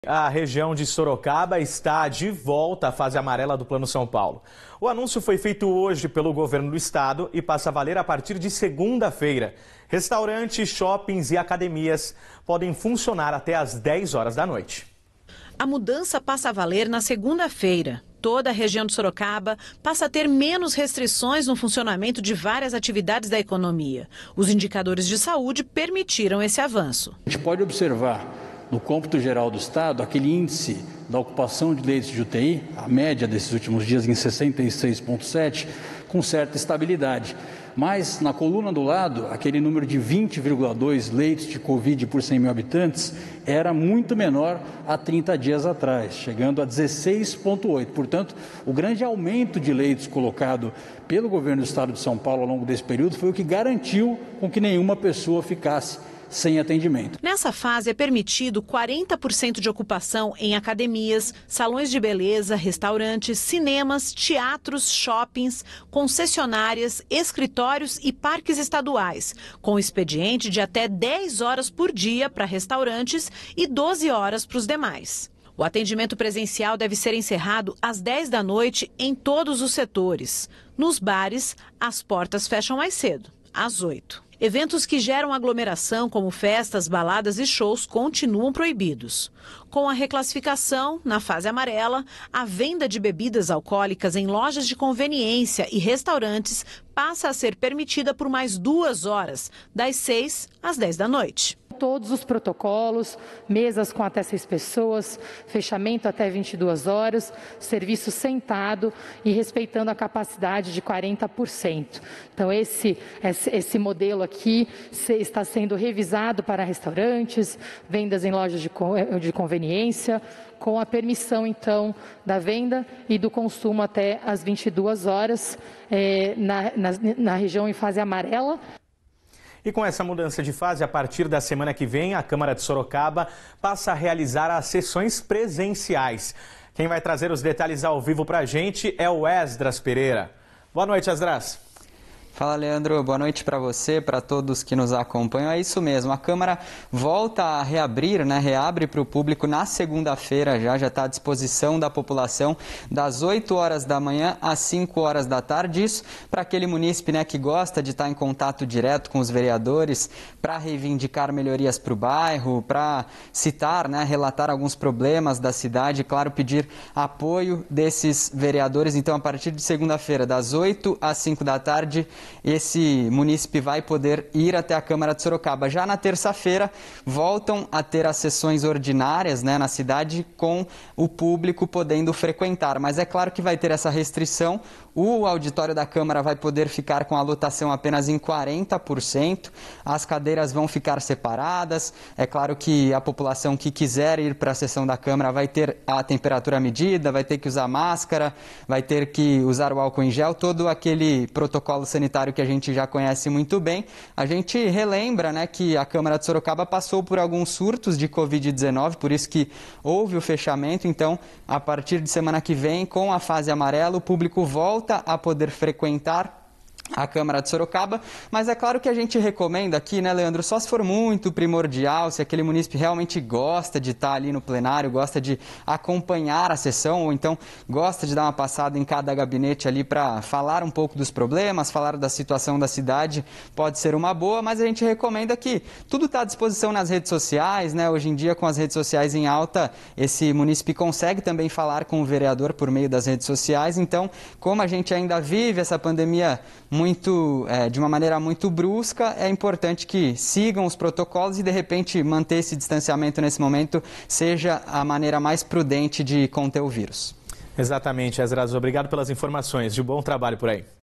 Região de Sorocaba avança para a fase amarela do Plano São Paulo | TV Sorocaba SBT
Região de Sorocaba avança para a fase amarela do Plano São Paulo.

O Governador João Doria anunciou nesta sexta (19) a 22ª reclassificação de regiões no Plano São Paulo de controle da pandemia. Baseado nos dados da saúde, Sorocaba passou para a fase amarela.